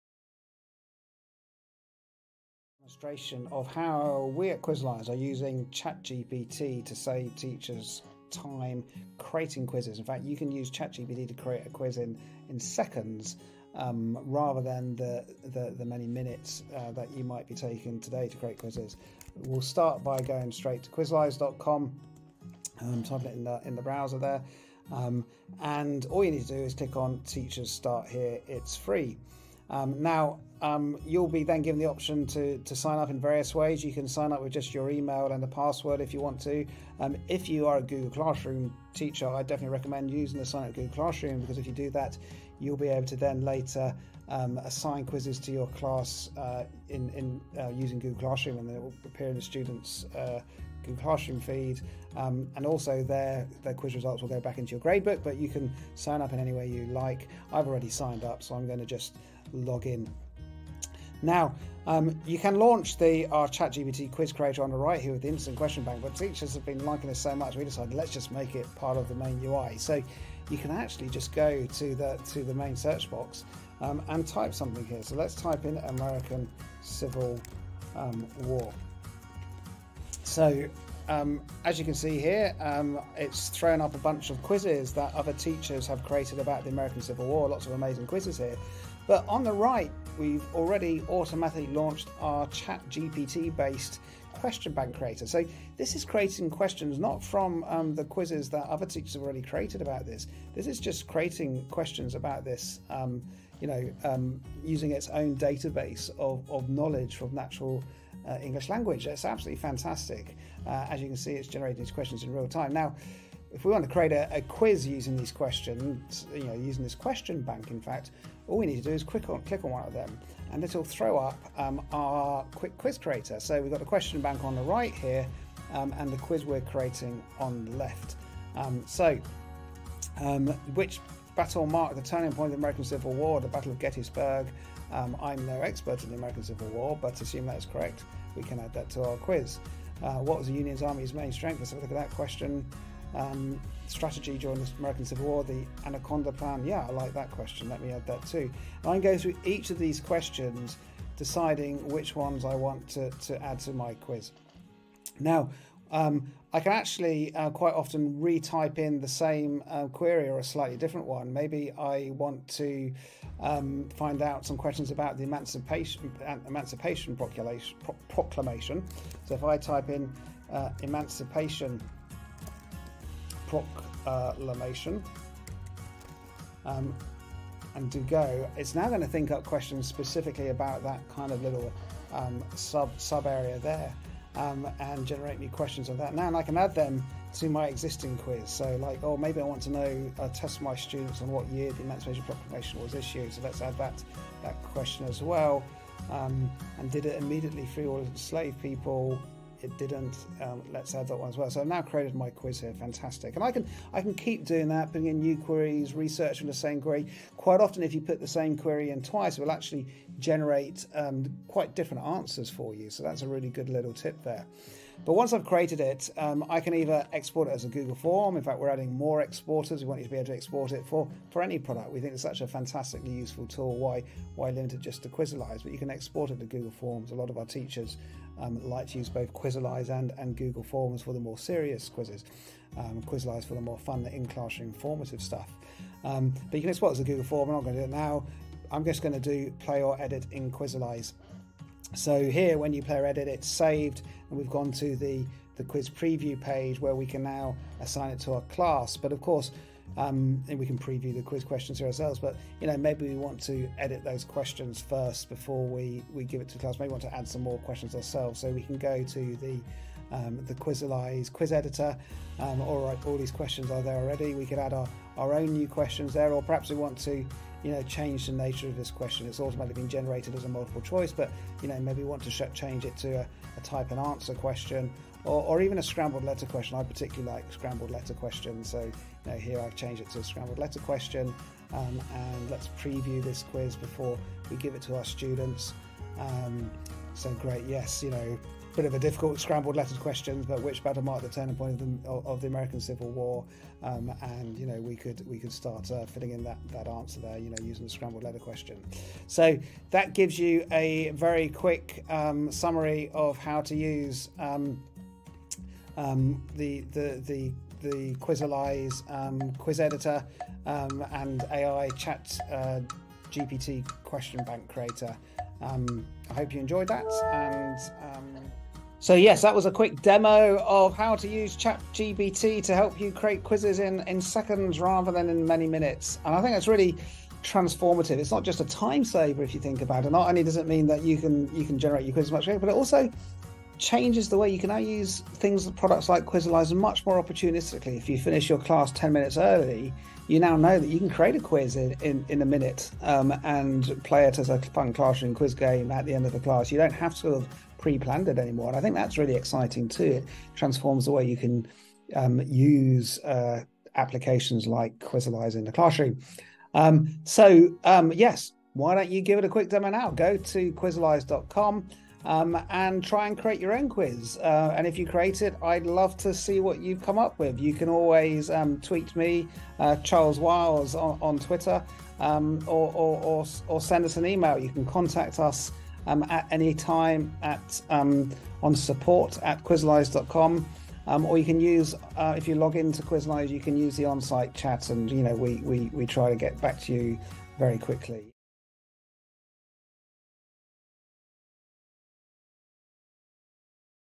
ChatGPT for Teachers - How to create a quiz in class without planning it
ChatGPT is here at Quizalize.com! Teachers can create quizzes in seconds with the power of AI, and assign them to their classes. Quizalize can save you a ton of time with our new ChatGPT Quiz Creator.

FAQ
-Is it expensive?
No, it's totally free. Create a free account at Quizalize now.

-Do I need to know how to use ChatGPT?
No, Quizalize does all the heavy lifting for you.

-Can I create a quiz in other languages?
Sí, claro! Yes, you can.